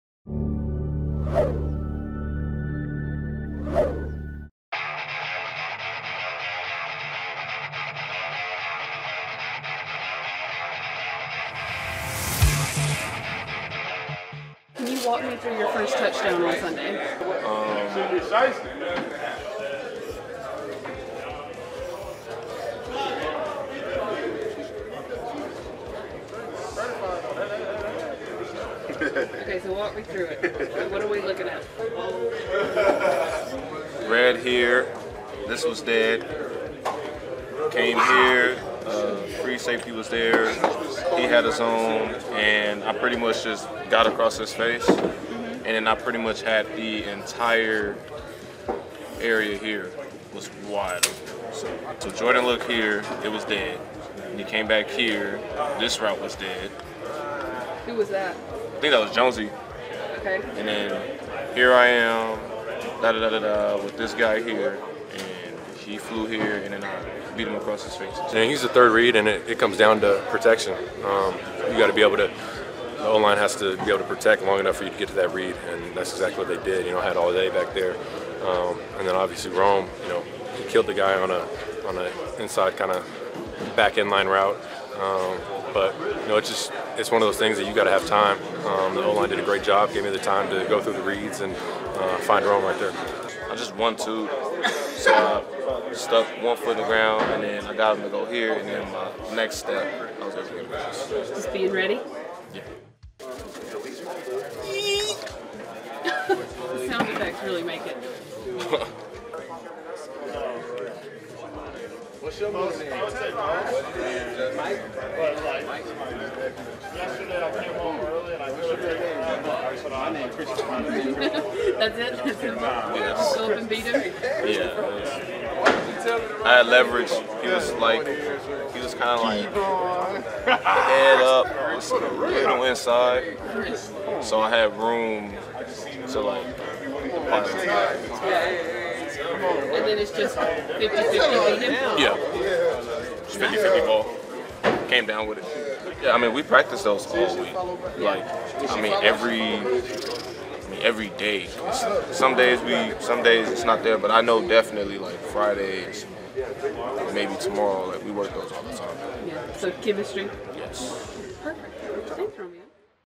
Chalk Talk: Romeo Doubs breaks down his two touchdowns in the Packers Week 1 win over the Bears
This edition of Chalk Talk sits down with Romeo Doubs to break down his two touchdowns in the Green Bay Packers Week 1 win over the Chicago Bears.